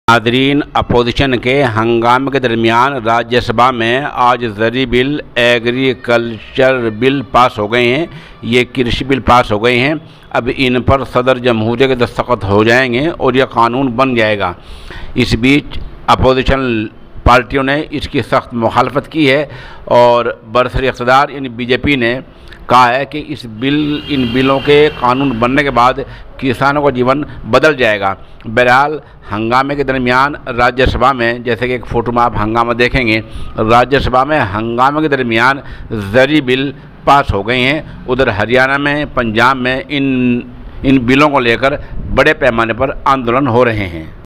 راجیہ سبھا میں بل پاس राज्यसभा में कृषि बिल पास
हंगामे के बीच राज्यसभा में कृषि बिल पास हो गए